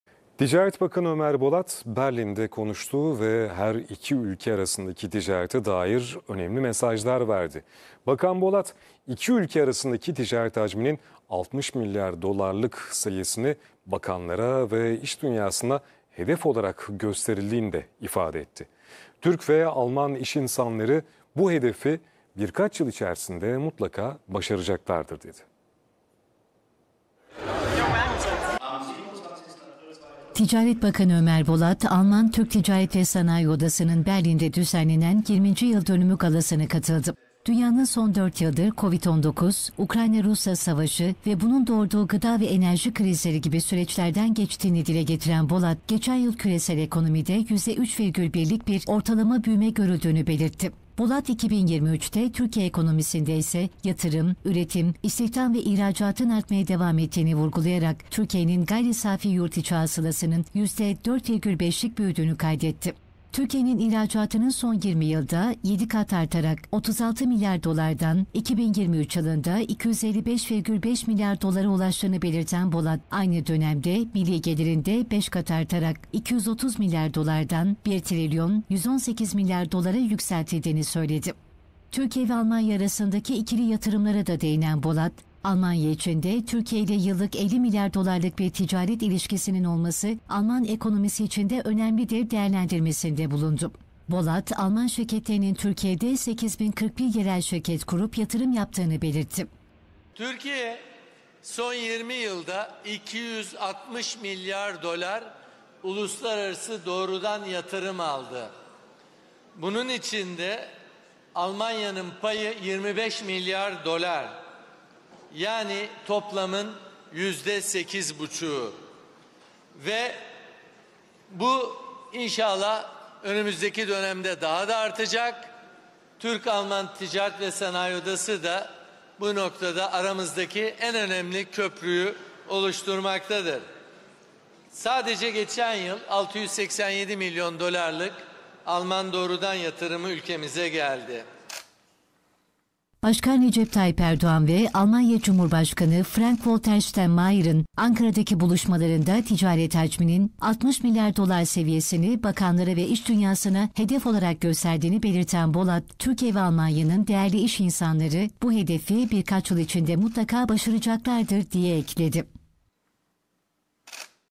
Bakan Ömer Bolat: Türk Ve Alman İş İnsanları 60 Milyar Dolar Ticaret Hacmini Birkaç Yılda Başaracak
Ticaret Bakanı Ömer Bolat, Başkan Recep Tayyip Erdoğan ve Almanya Cumhurbaşkanı Frank-Walter Steinmeier'in 4 gün önce Ankara'daki buluşmalarında iki ülke arasındaki ticaret hacminin 60 milyar dolar seviyesini bakanlara ve iş dünyasına hedef olarak gösterdiğini belirterek, “Türkiye ve Almanya'nın değerli iş insanları bu hedefi inşallah birkaç yıl içinde mutlaka başaracaklardır.” dedi.